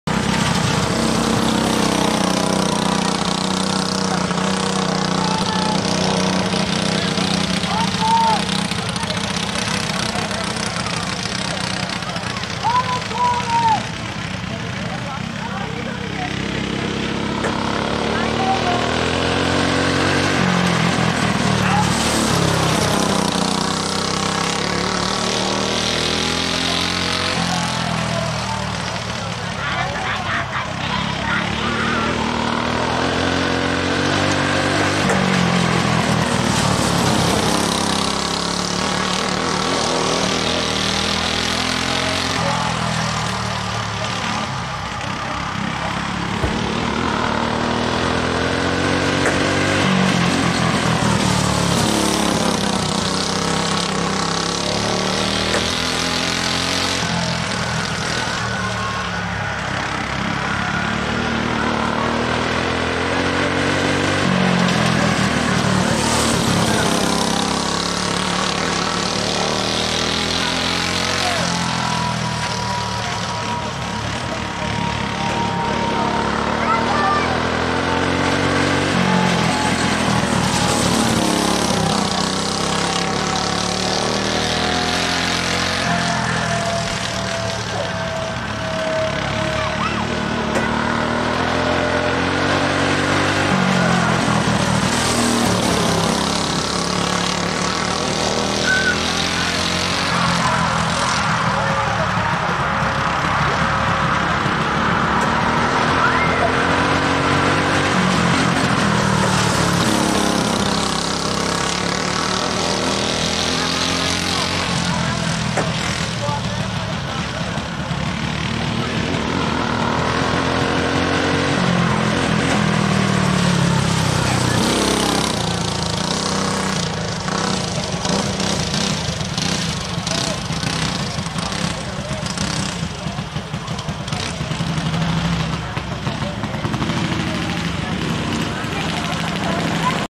Cage Kart Feature
Cage Kart Feature River Bottom Raceway Carlisle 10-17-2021